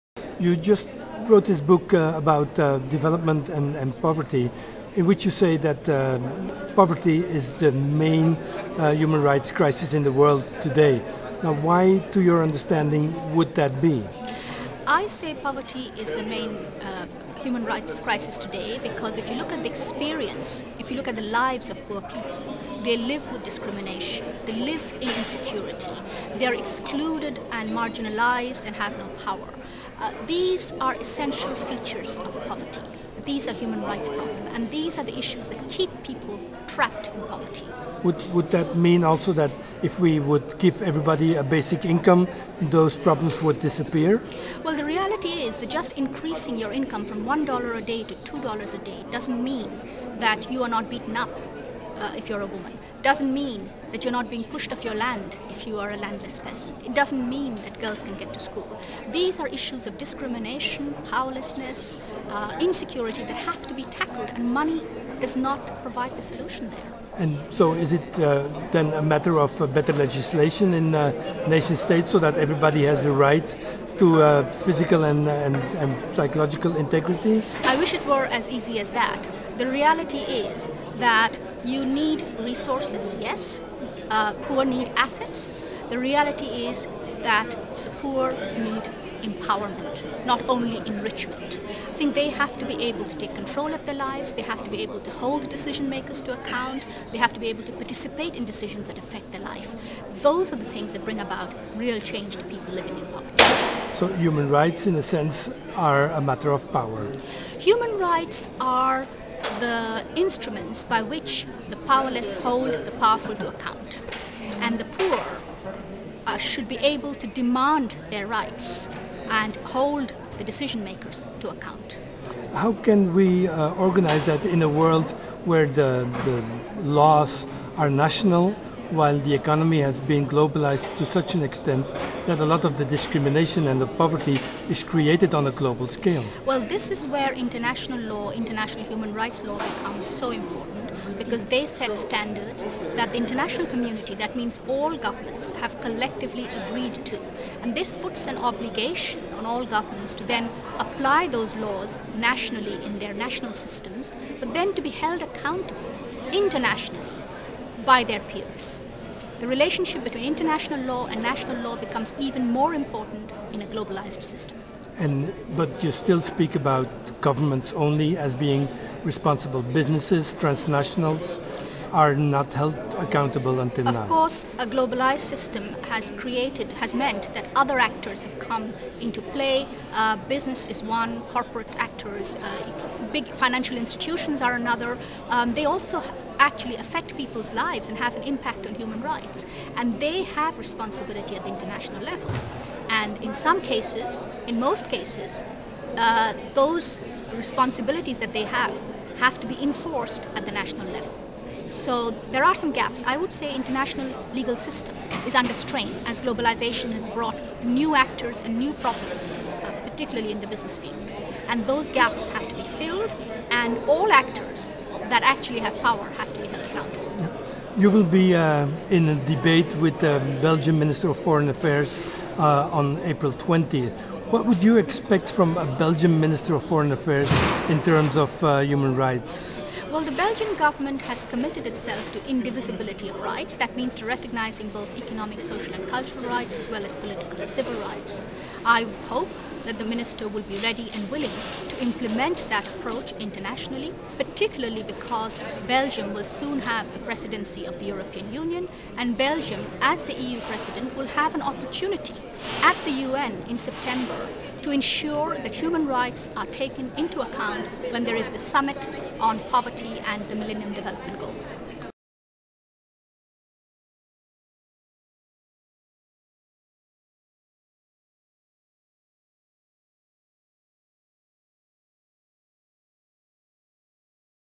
Irene Khan over mensenrechten
MO*lezing 'Armoede is de ergste mensenrechtencrisis' met Irene Khan en minister Steven Vanackere op dinsdag 20 april, Beursschouwburg Brussel, 19u30.